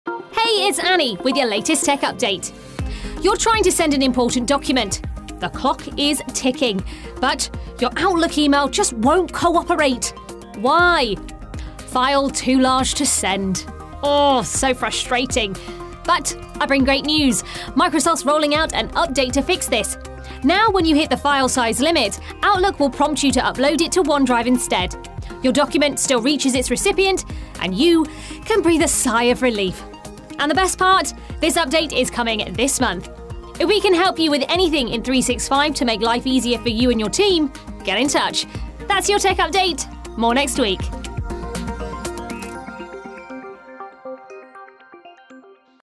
Weekly Tech Update 12th Sept 2023: End of the problem 'File Too Large to Send'
"Solving the 'File Too Large to Send' Problem with Microsoft's OneDrive Integration - A Game-Changer for Outlook Users!"

Are you tired of the constant struggle with the "file too large to send" error in your emails? Well, the good news is that your days of frustration are about to be over!

Microsoft has just made a groundbreaking announcement that will revolutionize the way you send files via email. In this video, we'll dive into the details of Microsoft's plan to integrate Outlook with their powerful OneDrive cloud storage platform.

Imagine never having to worry about attachment size limits again. With this integration, Outlook will seamlessly prompt you to upload your large files to OneDrive, ensuring your important documents reach their intended recipients without a hitch.

The best part? This is not some distant dream—it's happening soon! Android users are the lucky first recipients of this feature, with a broader rollout scheduled for this month. The wait is almost over!

Say goodbye to the tedious tasks of compressing, resizing, or resorting to potentially risky third-party services. Microsoft is paving the way for a smoother and more efficient email experience for all.

If you want to make the most of your Microsoft 365 subscription and stay ahead of the curve with this exciting update, don't hesitate to reach out. Your hassle-free email experience is just a click away!